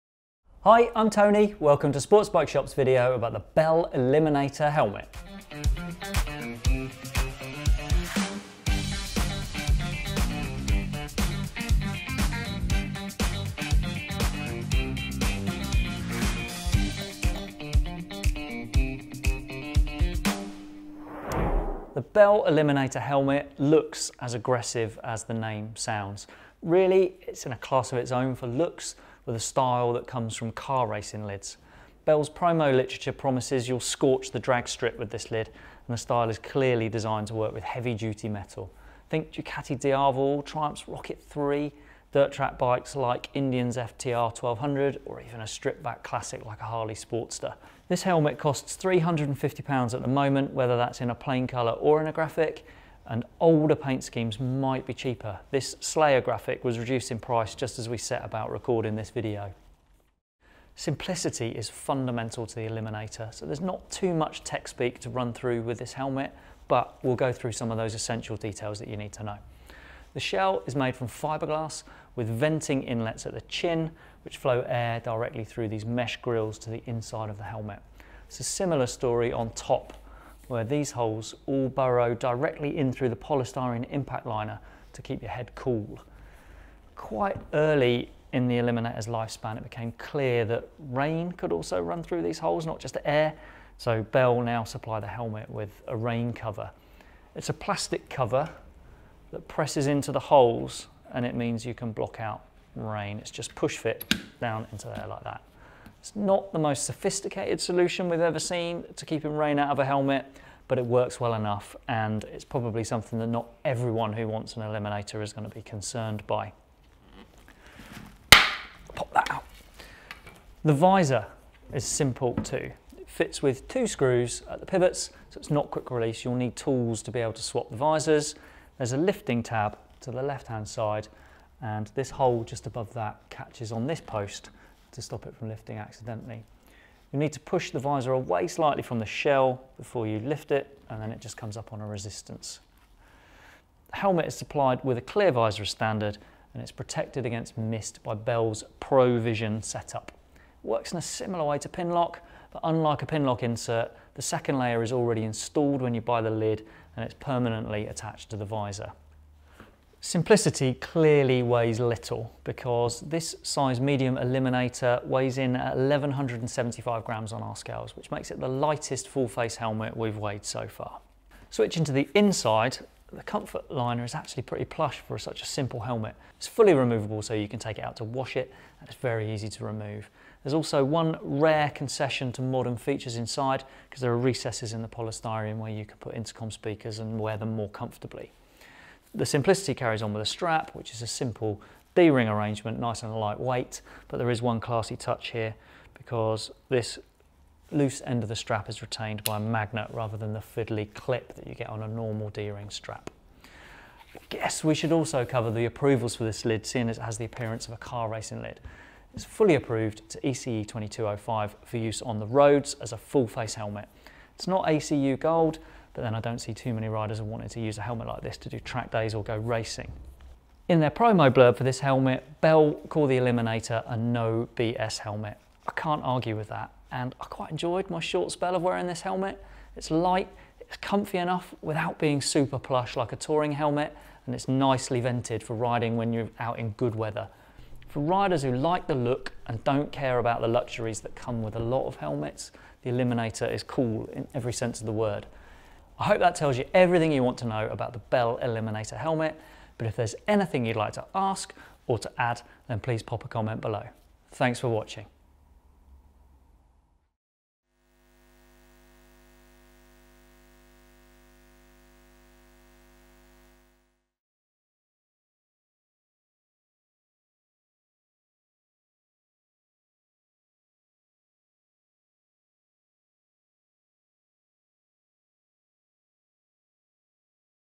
Bell Eliminator motorcycle helmet review - Sportsbikeshop
If anyone has the right to mimic the design of a car racing helmet then it’s Bell. They invented the modern protective helmet, and this Eliminator lid is a current-day version of those mean-looking lids. Get the complete picture on this heavy metal motorcycle lid with our video...